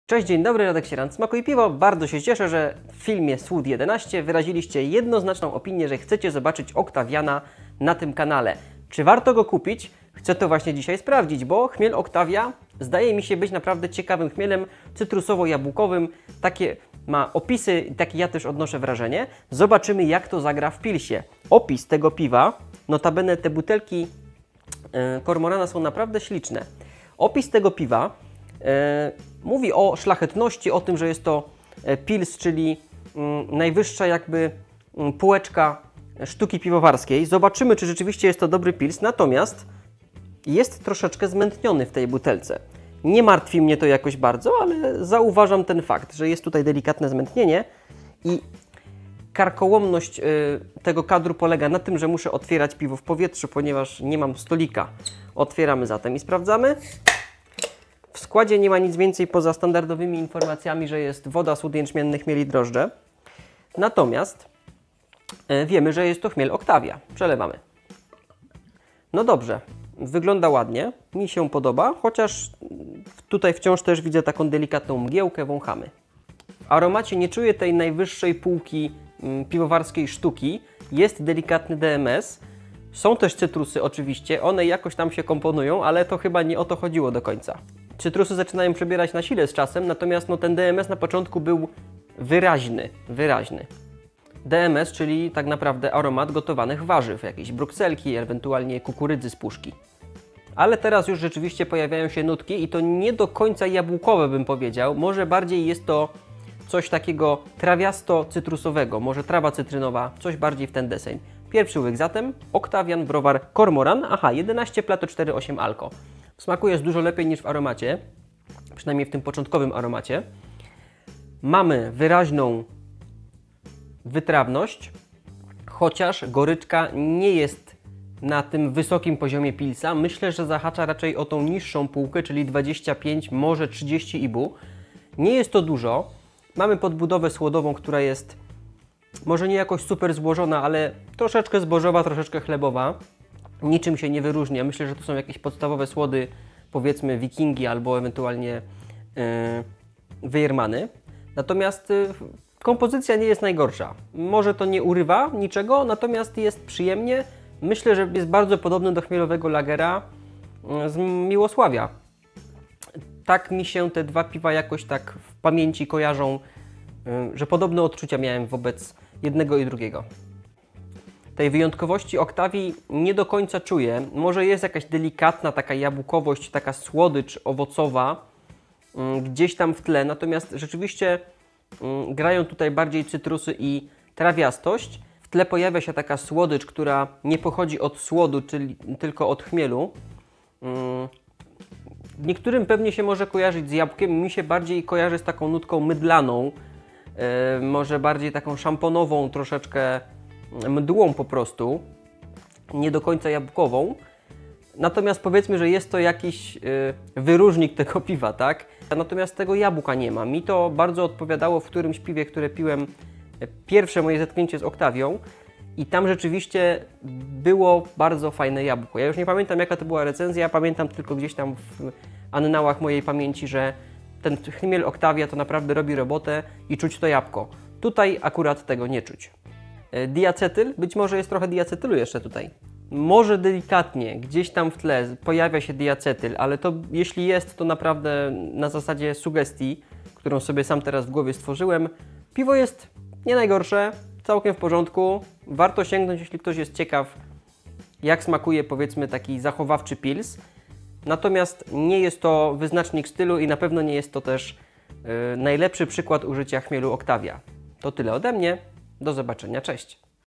Oktawian - browar Kormoran [Żabka]
Podwójnie chmielony chmielem Oktawia pils z browaru kormoran dostępny w Żabkach smakuje, no właśnie jak? o tym w tym filmie!